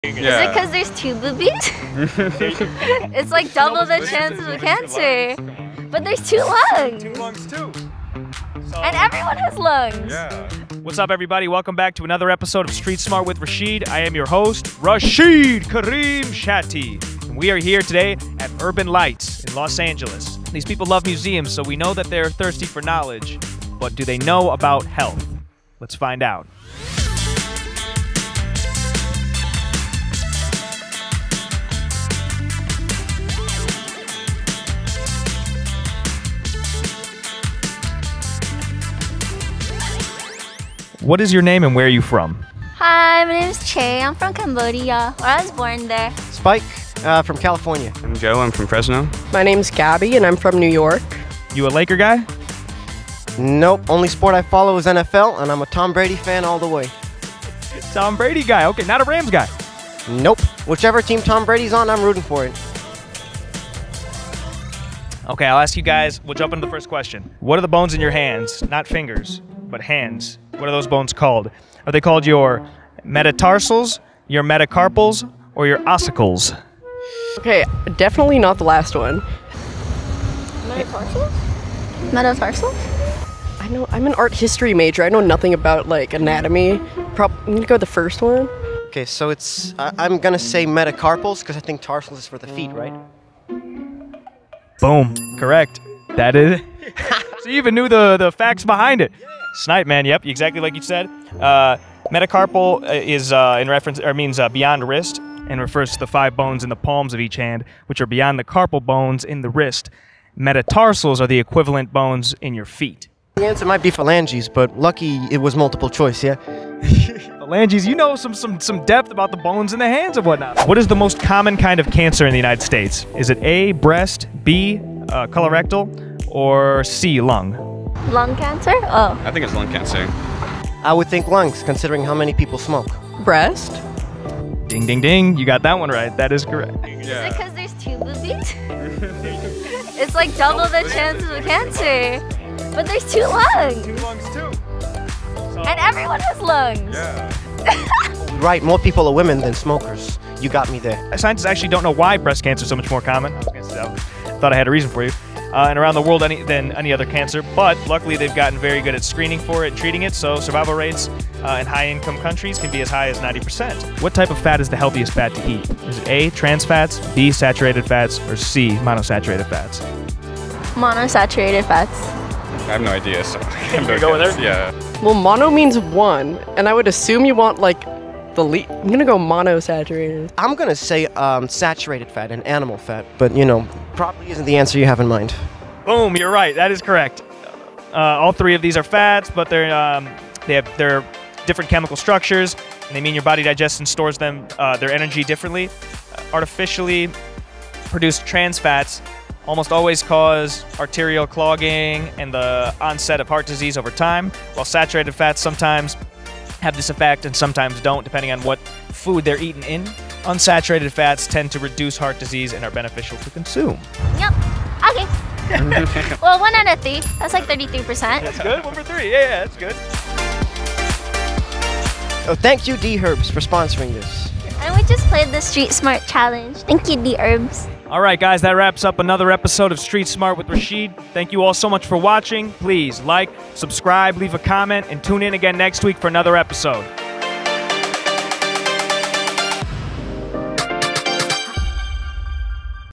What fat is the healthiest to eat?
We're putting art fanatics to the test!  Today we go to the LACMA, and quiz the local people as well as the tourists. How many questions will they get correct? Join us and let's see how well the people of Los Angeles know HEALTH! Did you get any of the questions right? Let us know down below!